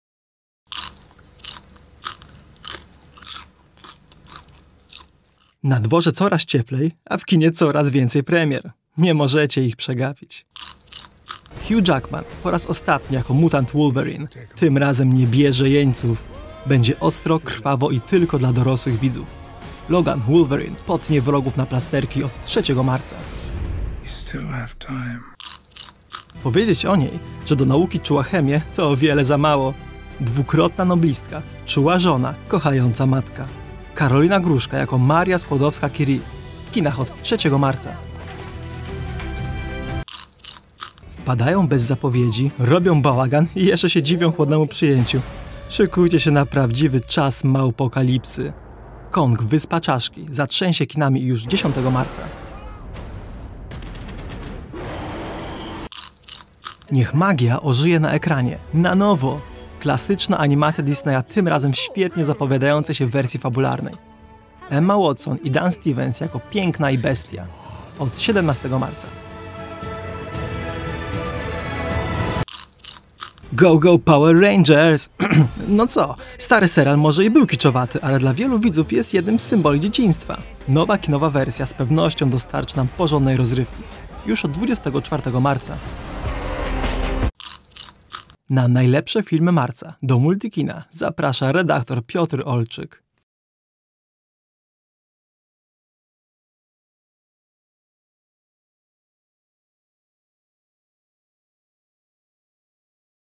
TOP Multikino #21 | Najlepsze filmy marca | 2017
O premierach filmowych w marcu opowiada redaktor Multikina – Piotr Olczyk. Zerknijcie na jego propozycje!